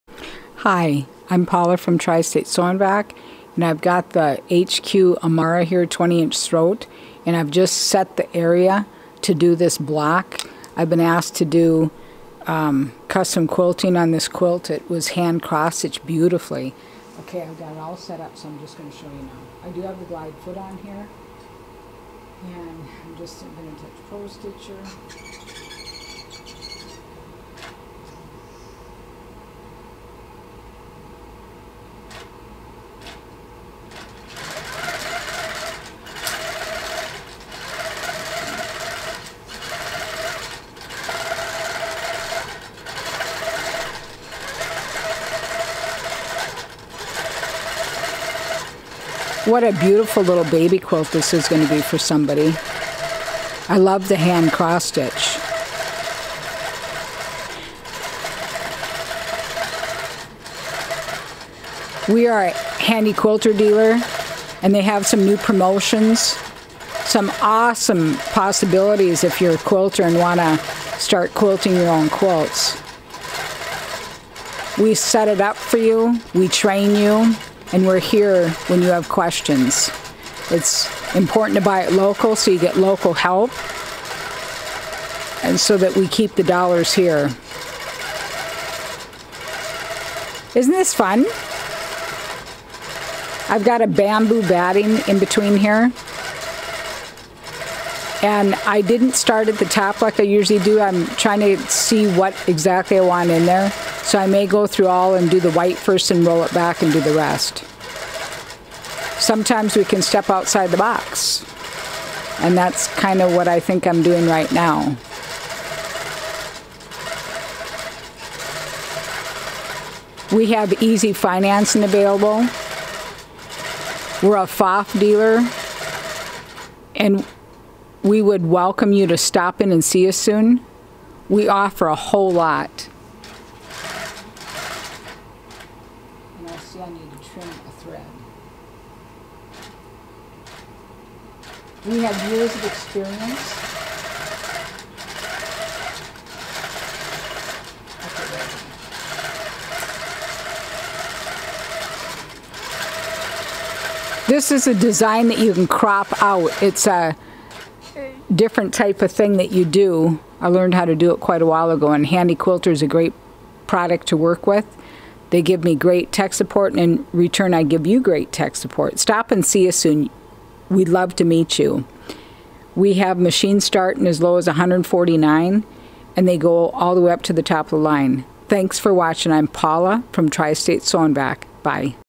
Paula quilting a cropped hand crossstitch teddy bear quilt block with HQ Amara & Pro Stitcher
Custom quilting a hand Cross Stitched Teddy Bears quilt by Paula Rudolph, Tri-State Sew & Vac., Inc. using the HQ Amara' 20" throat & Pro-Stitcher.